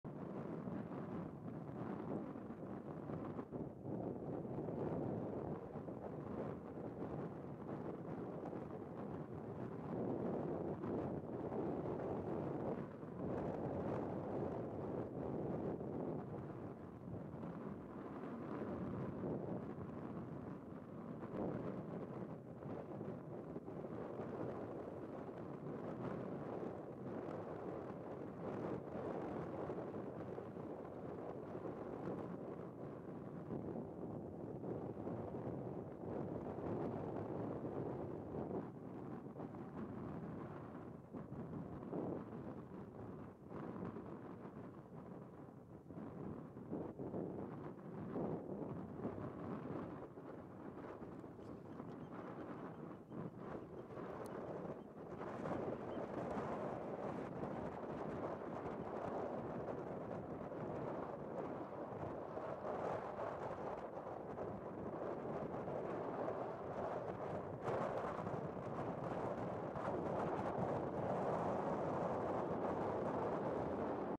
Austernfischer an der Nordsee macht großen Fang
Ein Austernfischer bei der Arbeit / Futter suche.
Er hat Glück und findet eine große Muschel.